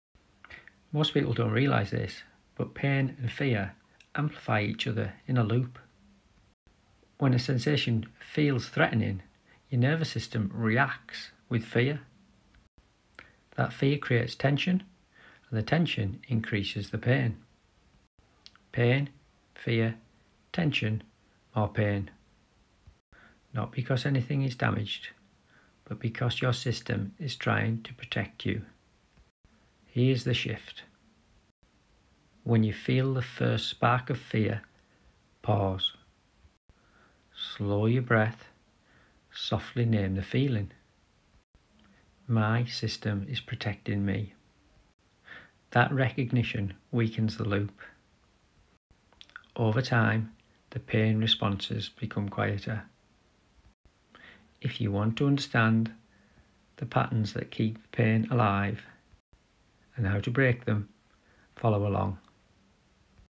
The Pain–Fear–Pain Loop Explained
Pain, fear, and tension often reinforce each other in a loop.
When a sensation feels threatening, the nervous system reacts with fear.
Fear increases tension — and tension increases pain.
This is protection, not damage.

When you catch that first spark of fear and soften your response,
the loop begins to weaken.